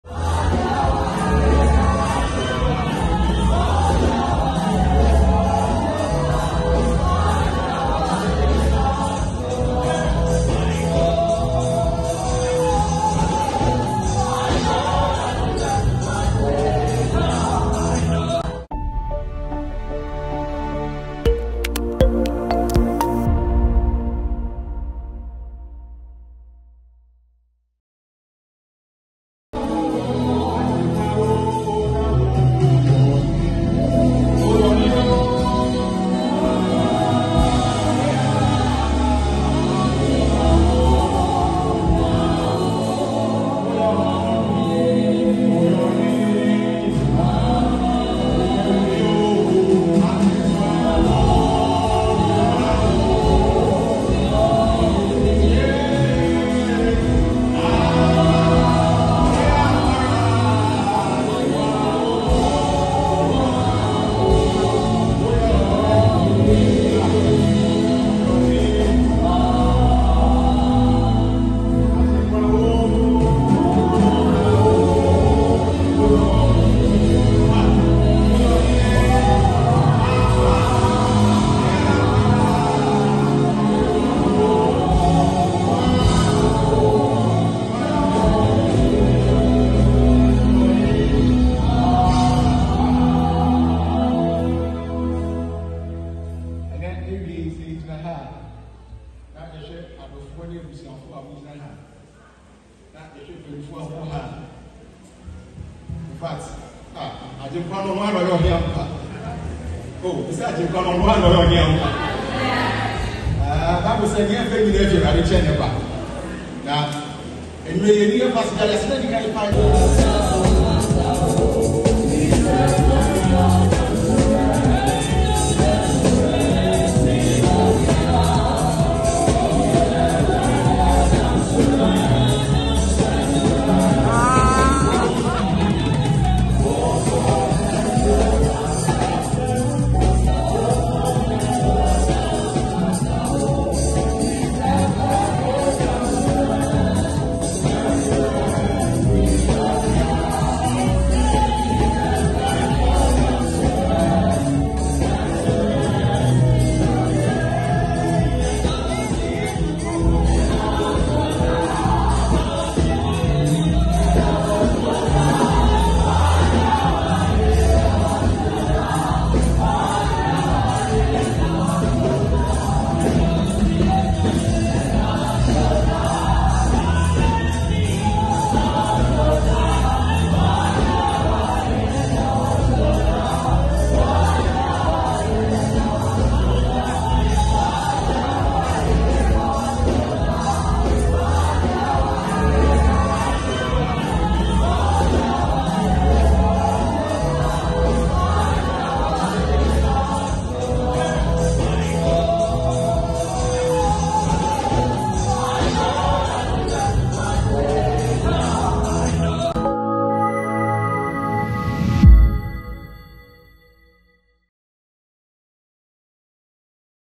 Boss joyfully takes the Dance floor at his wedding 💕
Congratulations to you Bra Banie, CEO of @christianvibestv on your wedding.